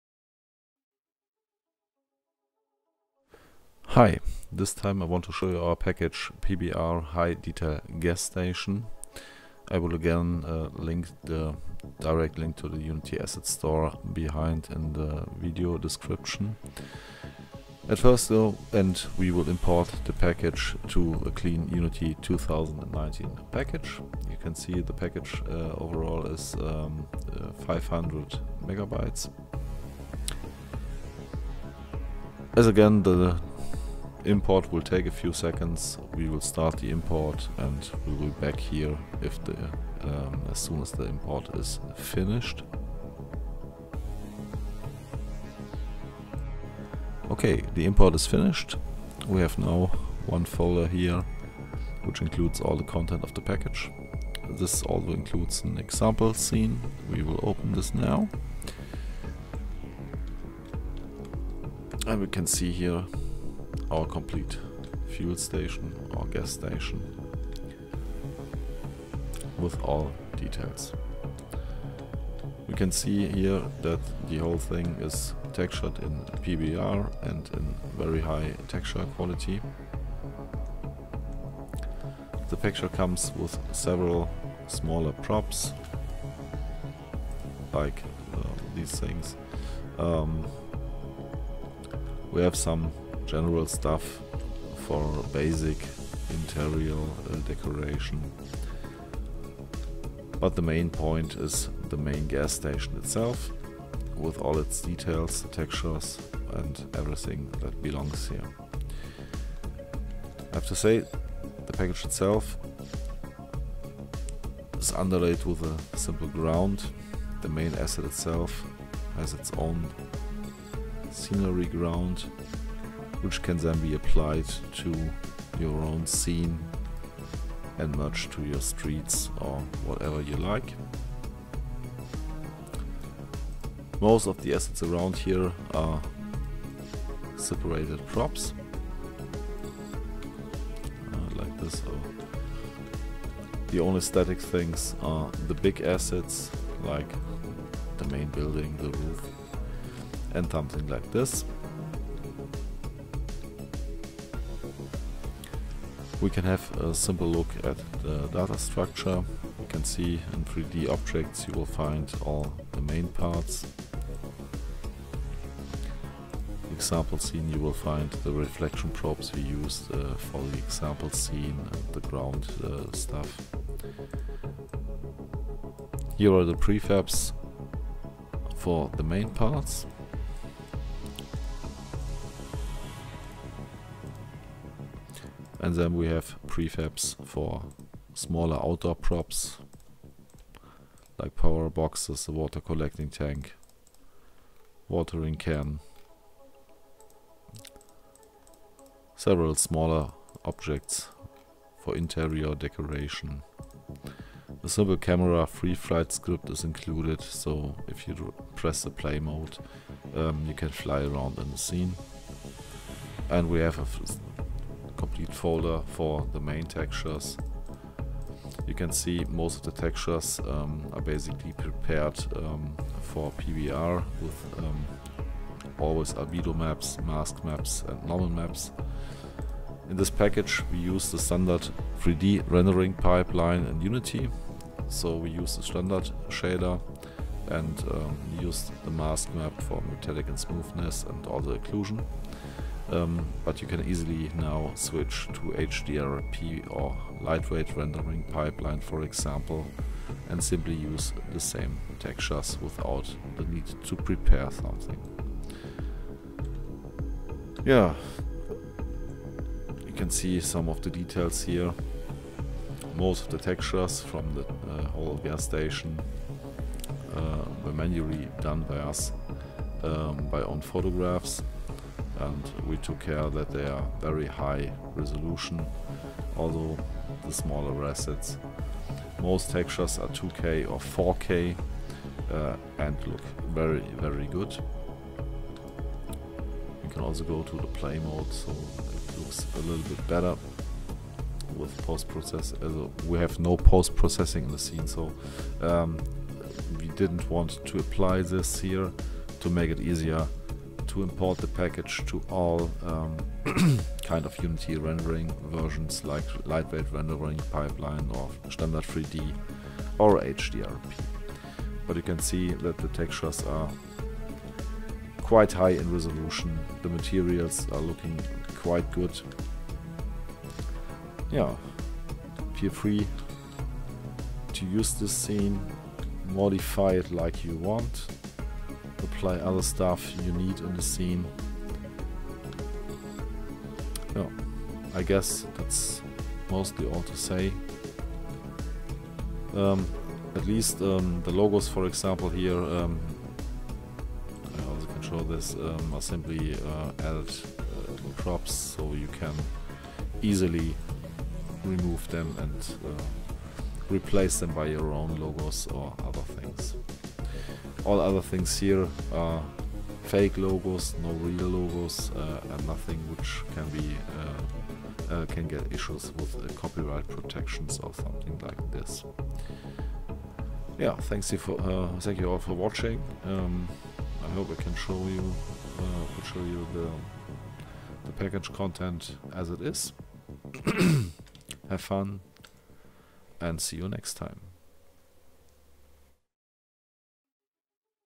AssetView - PBR High Detail Gasstation
In this video we show you our PBR High Detail Gasstation Asset for the Unity Asset Store.
This video shows the version 1.0!
The current version got a lot of updates!